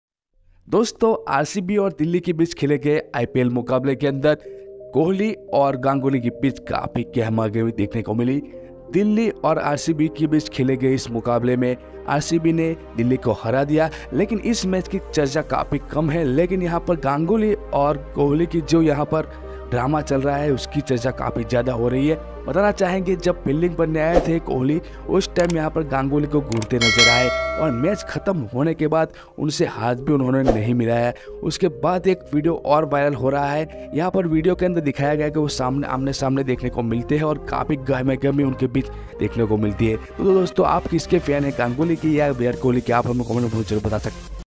Virat kohli angry on Sourav Ganguli हाथ नहीं मिलाया |IPL 2023
Virat kohli angry Sourav on Ganguli हाथ नहीं मिलाया |IPL 2023